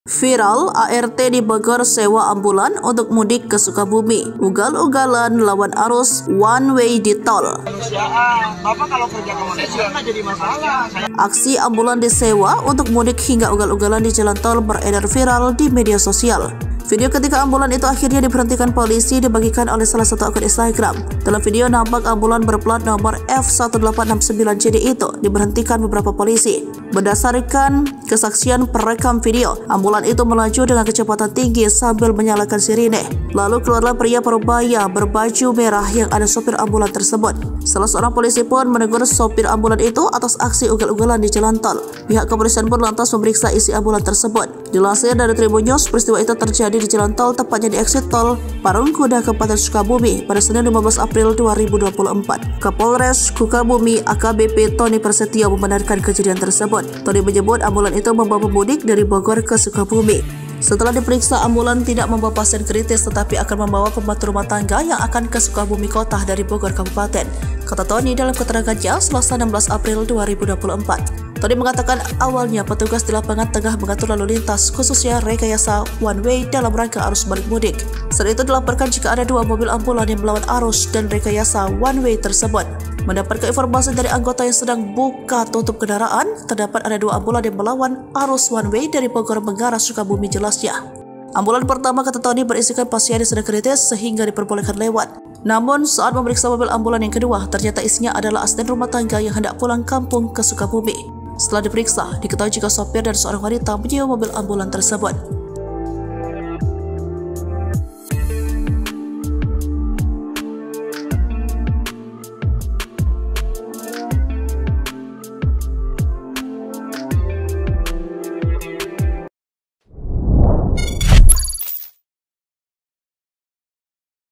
Viral ART di Bogor Sewa Ambulans untuk Mudik ke Sukabumi, Ugal-ugalan Lawan Arus One Way di Tol
Video ketika ambulans itu akhirnya diberhentikan polisi dibagikan oleh akun Instagram @kamerapengawas.id.

Dalam video, nampak ambulans berpelat nomor F 1869 JD itu diberhentikan beberapa polisi.

Lihat informasi terkini bersama kami, di:

Palembang.tribunnews.com

Sumber Video:  Instagram @kamerapengawas.id.
Editor Video: LSA
Vo: Fhelinda